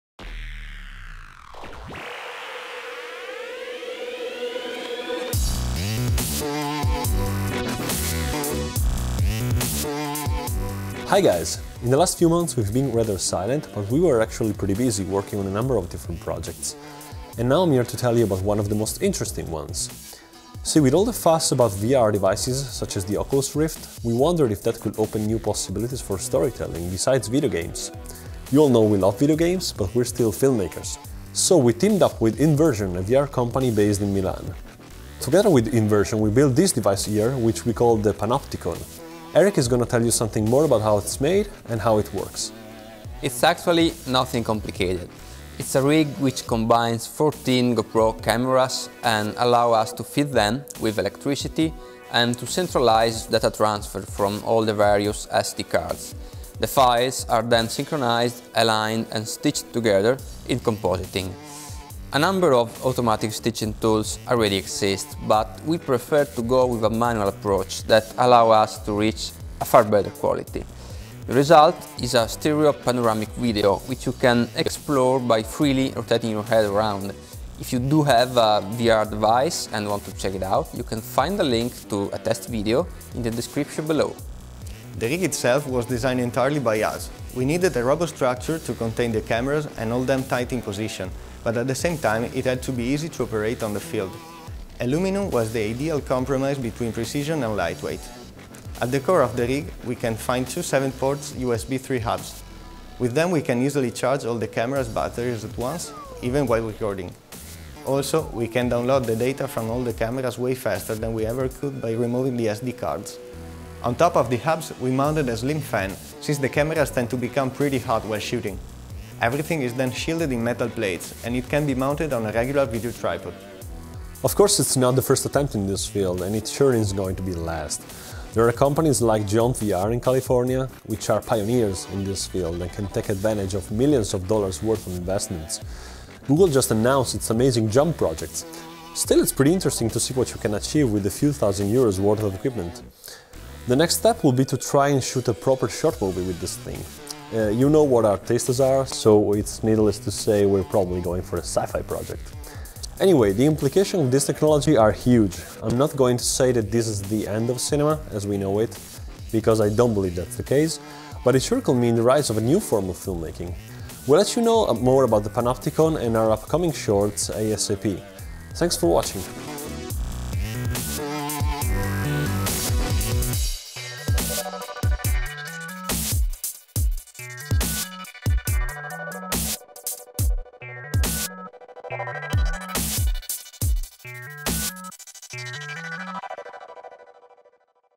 Introducing PanoptikonVR (DIY VR camera rig)
A 14-camera system that can create live action, 360°/3D video content (fiction, advertising, live events, e-learning).

PanoptikonVR has been created by Hive Division in a partnership with inVRsion.
Check out this demo clip with you VR device, if you have one!
(optimized for Samsung GearVR and Oculus Rift Devkit 2)